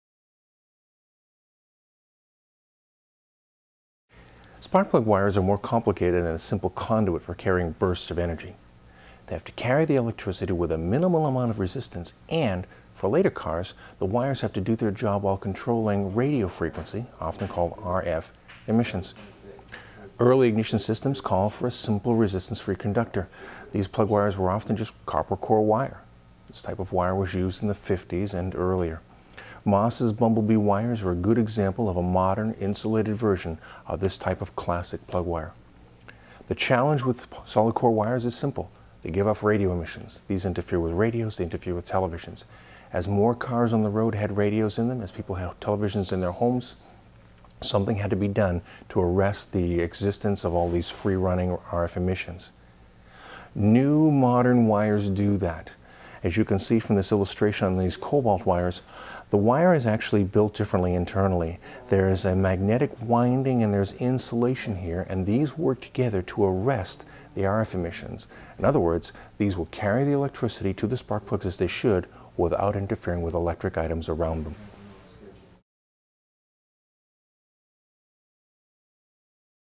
Ignition Wires - An Introduction
This video gives you an overview of spark plug wires and their function.